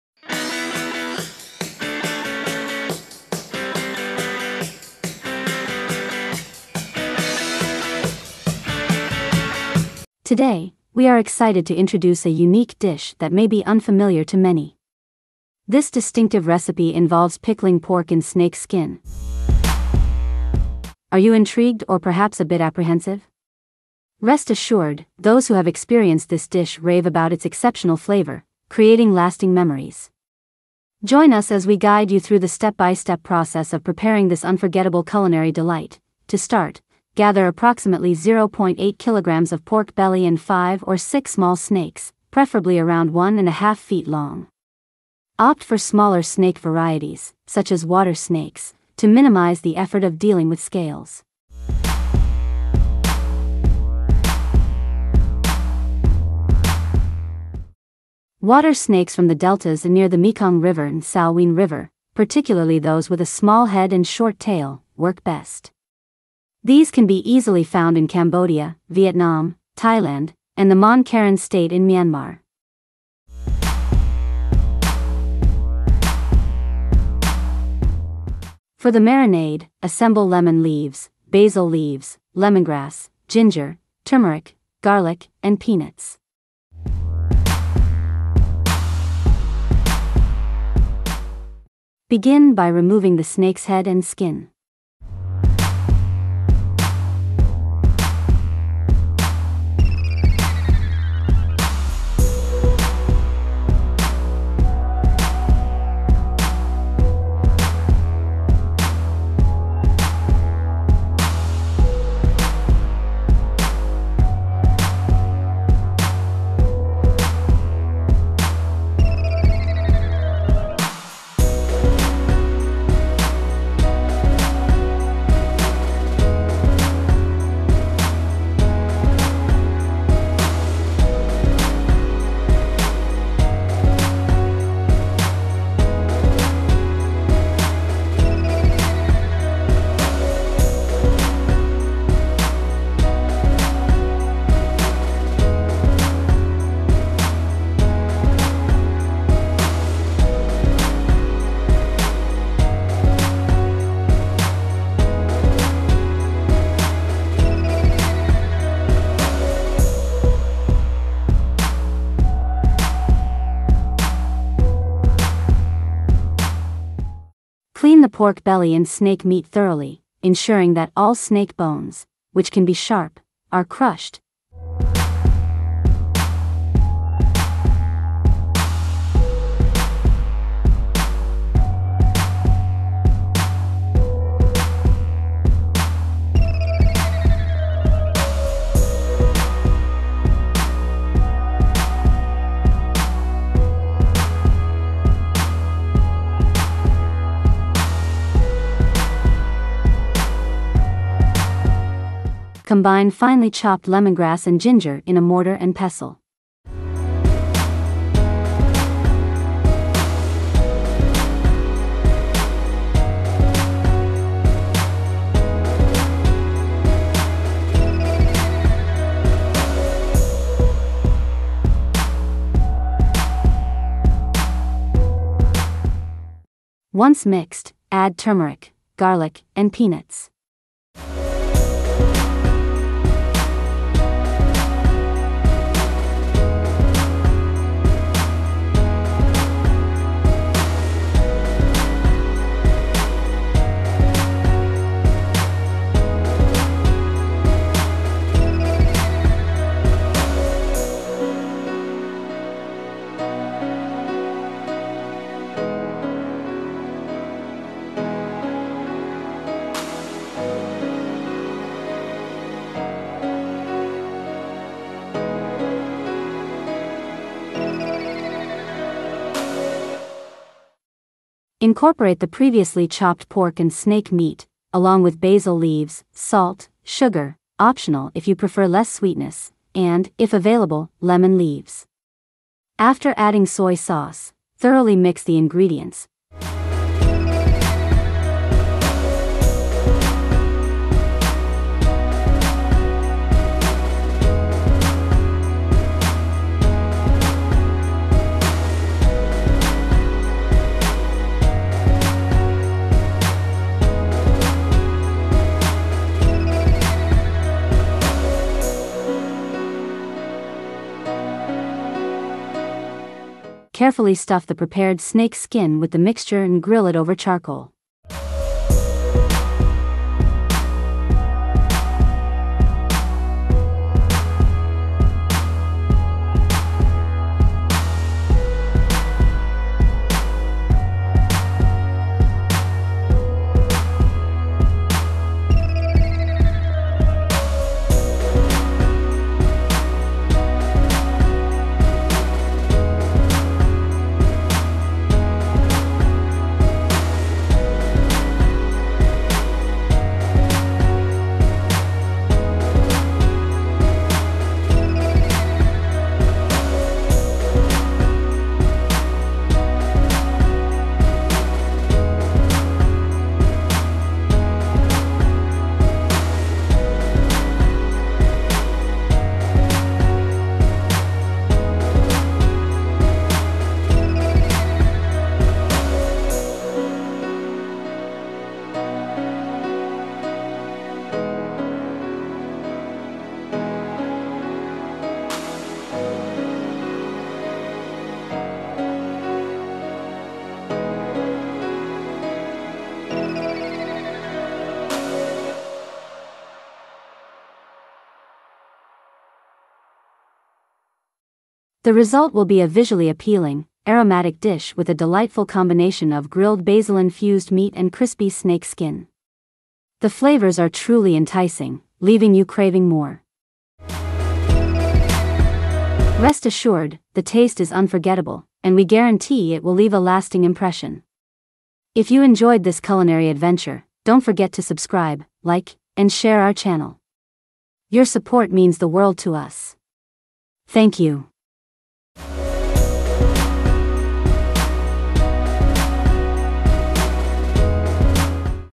Making A Scariest Dish: pickling pork belly in snake skin
we are excited to introduce a unique dish that may be unfamiliar to many. This distinctive recipe involves pickling pork belly in snake skin.

Are you intrigued or perhaps a bit apprehensive? Rest assured, those who have experienced this dish rave about its exceptional flavour, creating lasting memories. Join us as we guide you through the step-by-step process of preparing this unforgettable culinary delight.